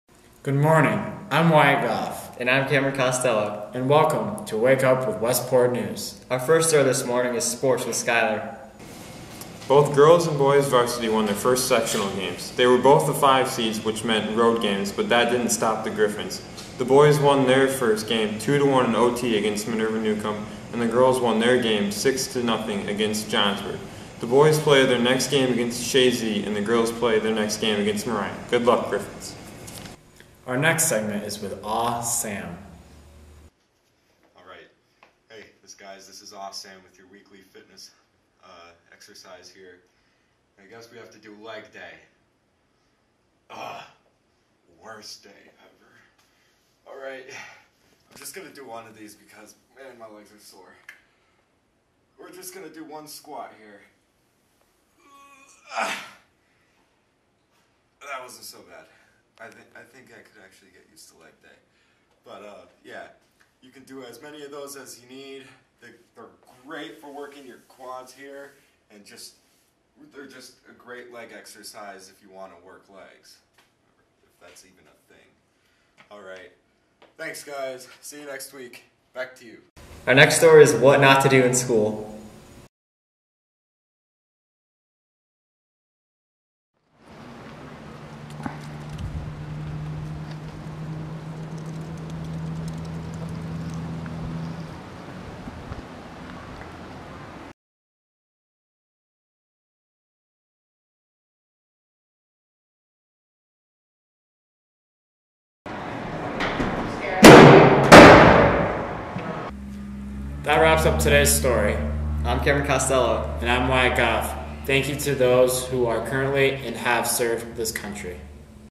Wake Up Westport 11_07_2016
Weekly news program for the week of November 7, 2016 at Westport Central School.